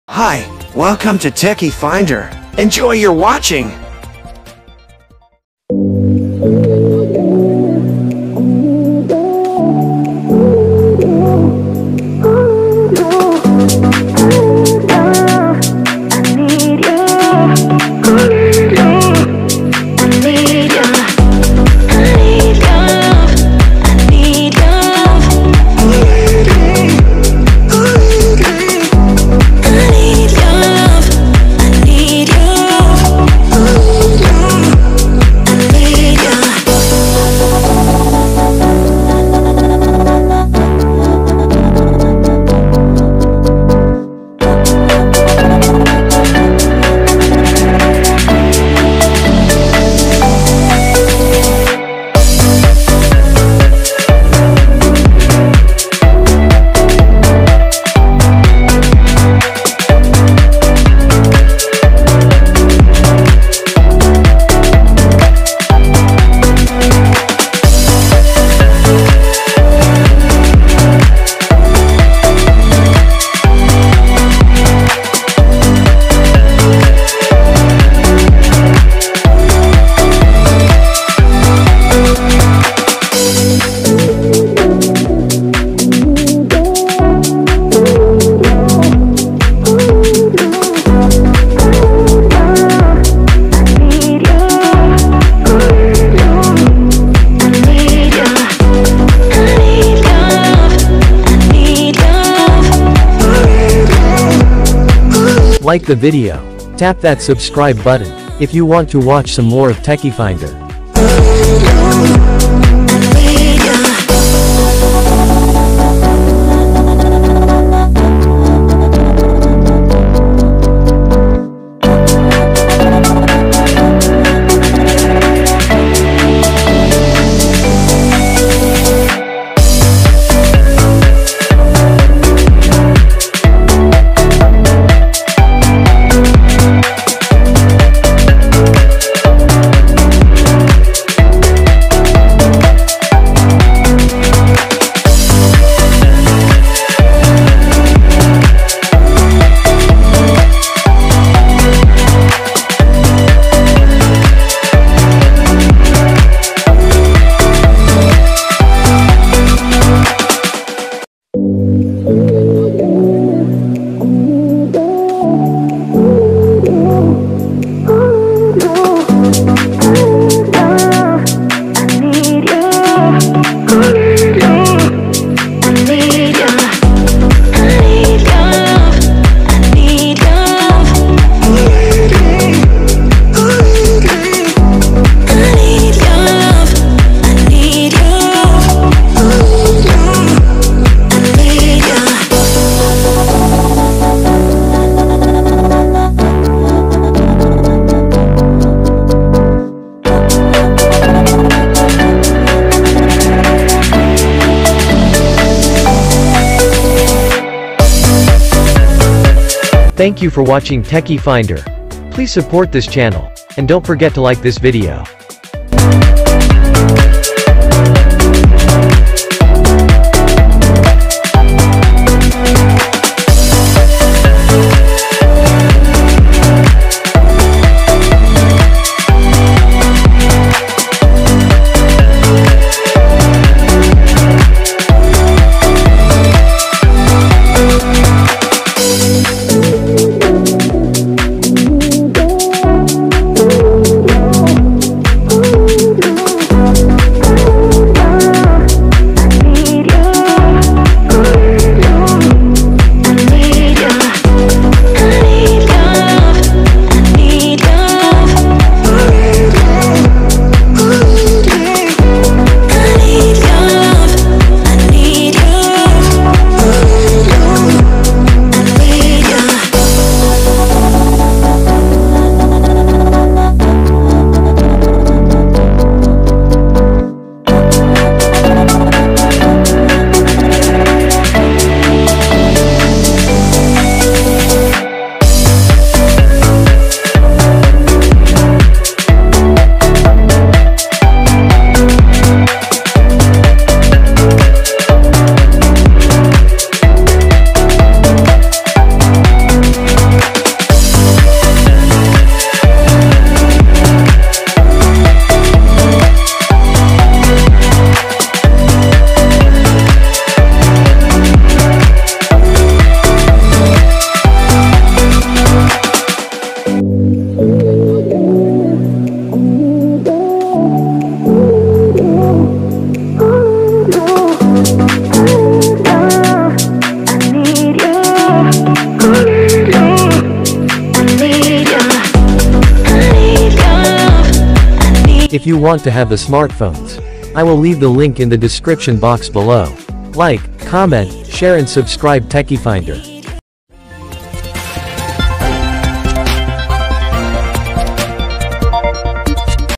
Tecno Spark 10 Pro vs Xiaomi Redmi 10 (2022) | Specification | Comparison | Features | Price
🤗 WHERE TO SHOP 🛒 👇👇👇

LAZADA LINK(S)

Also Check it out. In local retail stores near you.

Summary Specification

📱Brand: Tecno
👉Processor: Mediatek Helio G88
👉RAM: 8 GB
👉Storage: 128 GB, 256 GB
👉Display: 6.8 inches
👉Camera: Triple
🔋Battery: Li-Po 5000 mAh

📱Brand: Xiaomi
👉RAM: 4 GB, 6 GB
👉Storage: 64 GB, 128 GB
👉Display: 6.5 inches
👉Camera: Quad Camera